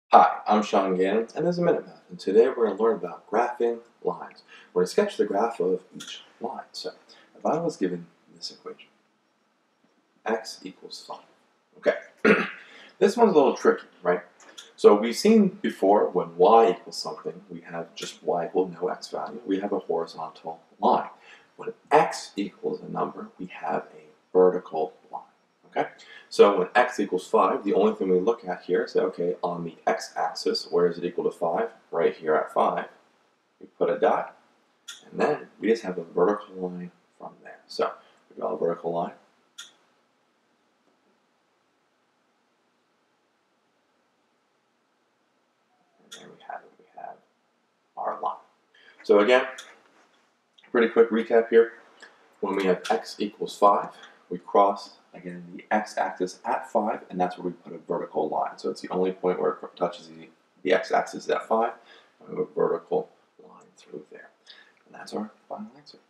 How to Graph x=5
In this math video lesson I show how to graph x=5. The equation in this video is in slope-intercept form, y=mx+b, and is a common way to graph an equation of a line.